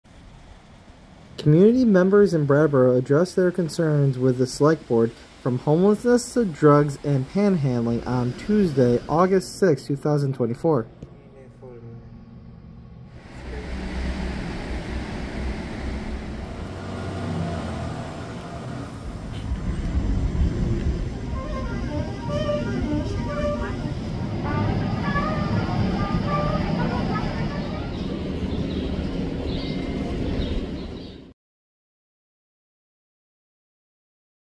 Community talks about concerns
Community members in Brattleboro address their concerns with the selectboard from homelessness, drugs and panhandling on Tuesday, Aug. 6, 2024.